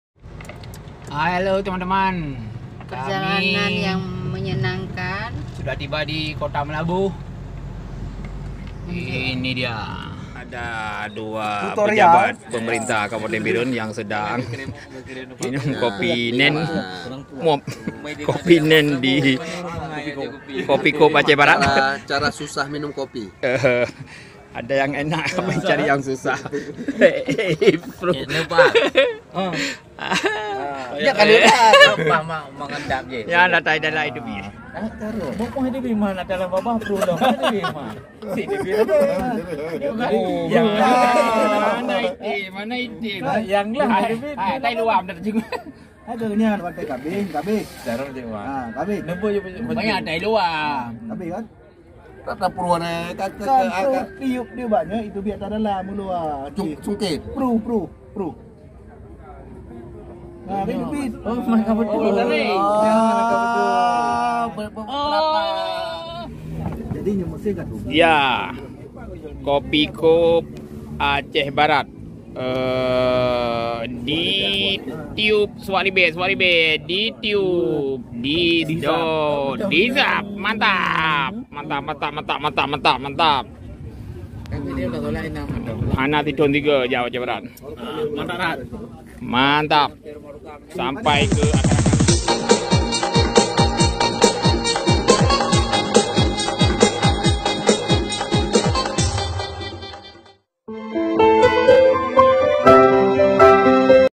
Cara Unik Minum Kopi I Kopi Khop Suwak Ribei Meulaboh
"Besok pagi kita akan minum kopi di Meulaboh atau aku akan mati syahid." Itulah kalimat yang diucapkan oleh Teuku Umar, pahlawan nasional asli Aceh sebelum memimpin penyerangan terhadap Belanda. Sayang, sebelum sempat menyerang, suami Cut Nyak Dien ini sudah lebih dahulu dibunuh Belanda.

Kata-kata Teuku Umar ini menjadi bukti bahwa masyarakat Aceh sudah sejak lama mengenal kopi. Selain karena kualitas kopinya yang sudah mendunia, Aceh juga memiliki Kupi Khop, suatu cara minum kopi yang unik. Lazimnya, minum kopi dilakukan dengan menyeruput dari bibir cangkir atau gelas. Namun di Aceh Barat, tepatnya di Meulaboh kopi disajikan dengan gelas terbalik di atas sebuah piring kecil.